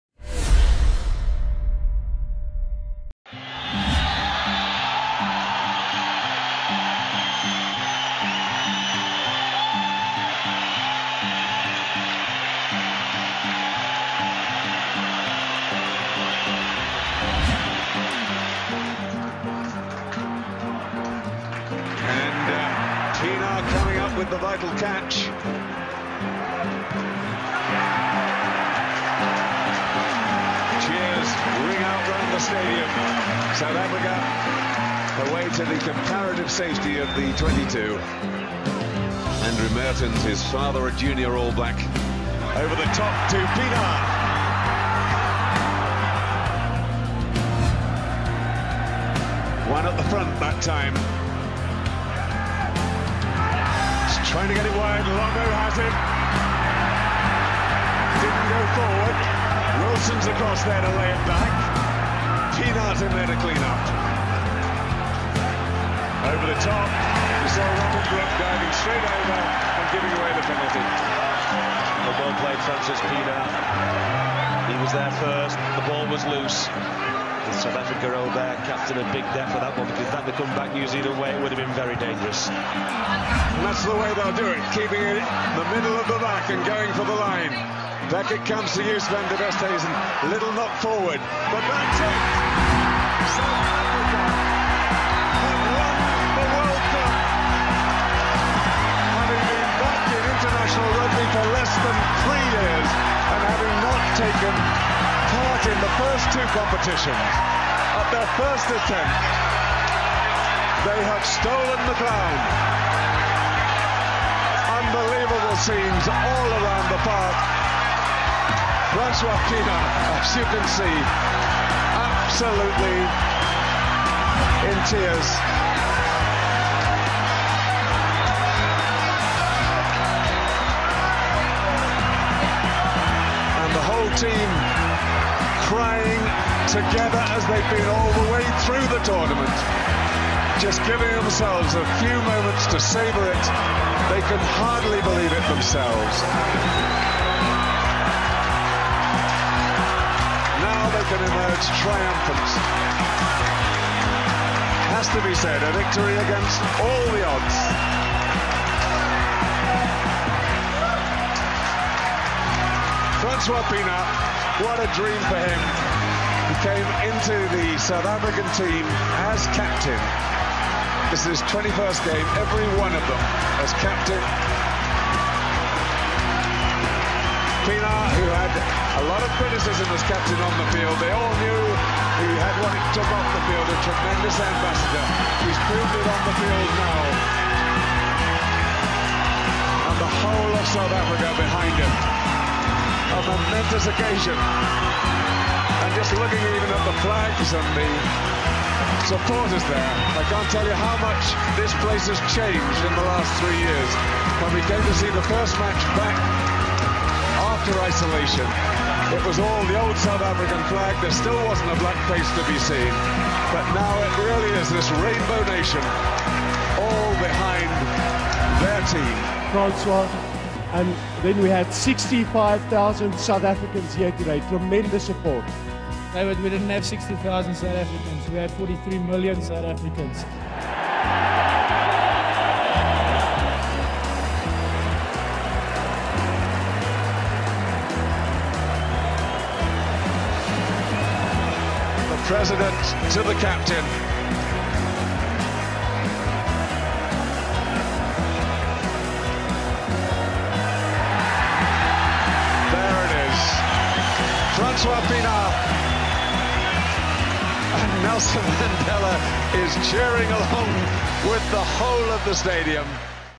Франсуа Пиенаар - легенда сборной ЮАР и Кубка мира по регби
1995 год, Нельсон Мандела победил Апартеид, но проблем внутри Южноафриканской республики еще очень много. Помочь в борьбе с ними должен Кубок мира, где впервые участвуют и впервые принимают который "Спрингбокс". Темнокожие и белые игроки в одной команде должны добиться успеха, а поведет их в бой капитан - Франсуа Пиенаар.